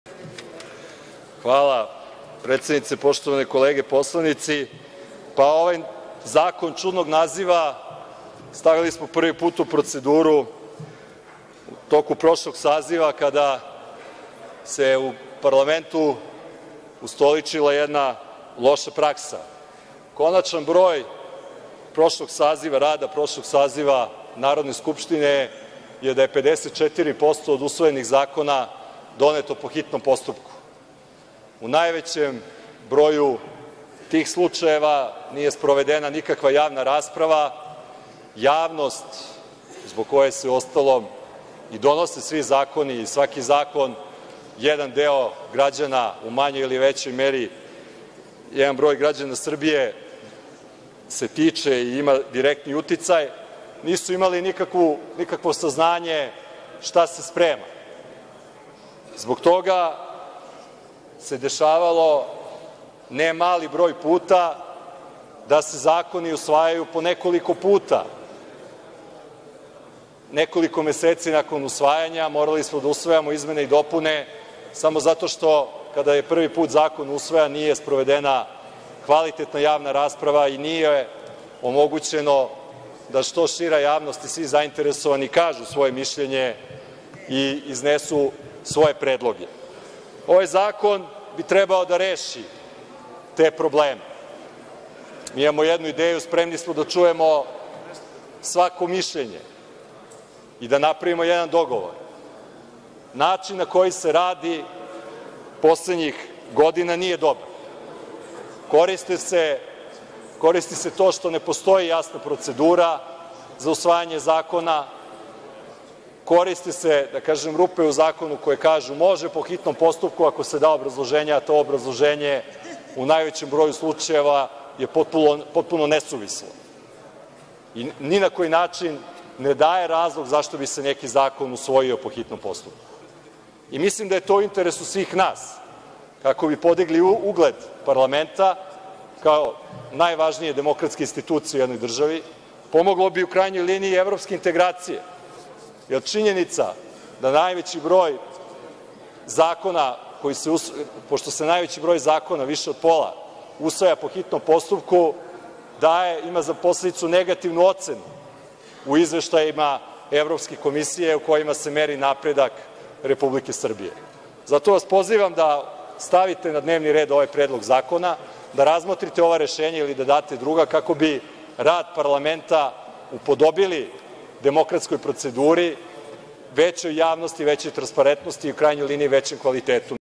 Marko Đurišić SDS - Predlog Zakona o predlaganju zakona (Oktobar, 2016.)
Narodni poslanik i šef poslaničke grupe SDS Marko Đurišić.

Beograd,
Oktobar, 2016.